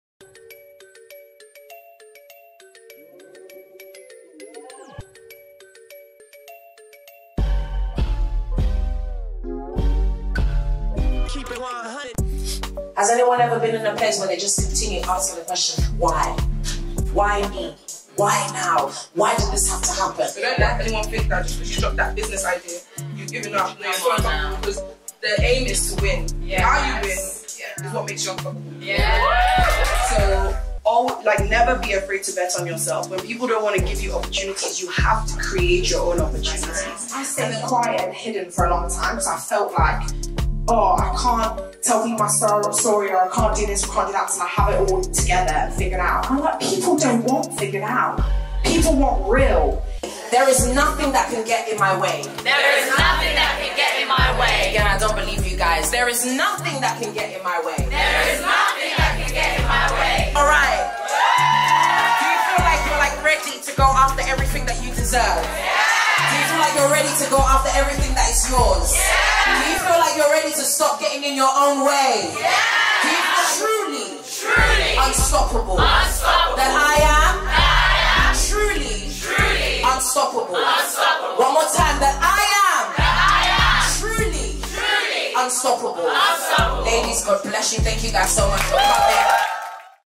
Our 2nd Annual Power Brunch SOLD OUT! | The Power Brunch 2023
A Time Was Had At Our 2nd SOLD OUT Annual Power Brunch!!! 😭🥂🥳🔥

On Saturday the ladies completely took over my favourite restaurant Deluxe Manna for our annual brunch & we had the most incredible time! We laughed, we cried, we ate GOOD food & most importantly we came together as like minded WOMEN and reminded each other of WHO we are and why we have to keep going! 🔥

If you were at the brunch let me know what your favourite moment was in the comments below! 😁⬇️

A massive thank you to all the people who without, none of this would have been possible 🫶🏽

Firstly our fabulous Power Panelists who were so vulnerable, so authentic AND who brought so much WISDOM to the conversation around what it meant to be an 'UNSTOPPABLE WOMAN' as per our theme for this year: Tiwalola Ogunlesi, Kezia Lawrence-Graham & Mamadinya - thank you! ❤️

🎈@loveteescreations on the amazing balloon decor
🍰 @nakabrownies on the incredible desert table
🎥 @p_mubenga on the crisp visuals
📸 @nubianella on the photography

And of course the entire @thepowerclass team who brought the vision together so BEAUTIFULLY! ❤️🔥

Next stop Power Brunch 2024 👀😜